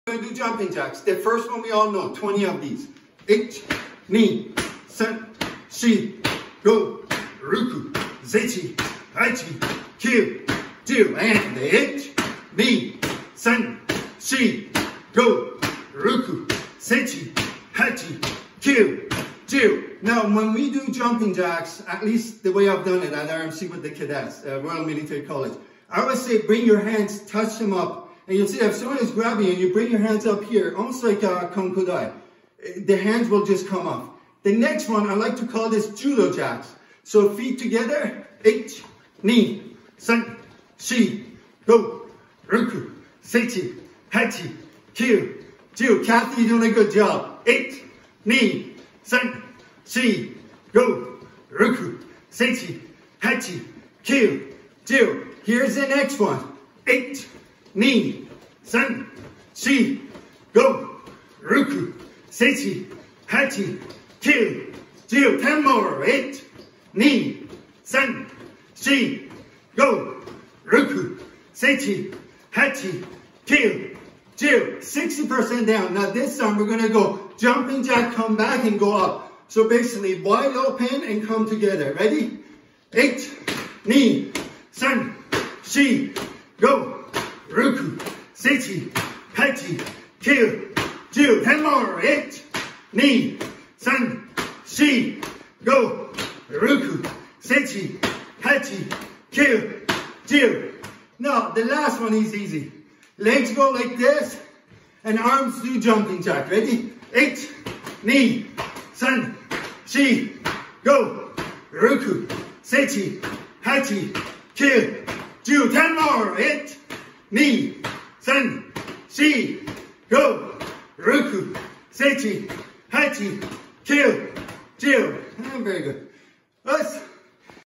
#56: Let’s do one Hundred jumping Jacks together (five different kinds)
Video from lesson taught on zoom, 7 February 2021. You can use this video to do hundred jumping jacks in each workout.